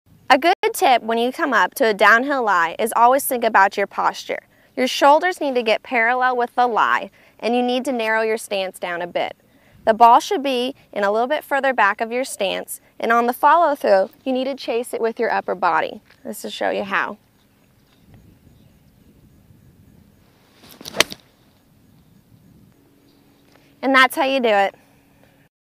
Paula Creamer Downhill Lie Golf Tip - Golfonline
Paula Creamer from TaylorMade shows how she plays a downhill lie shot - Golffonline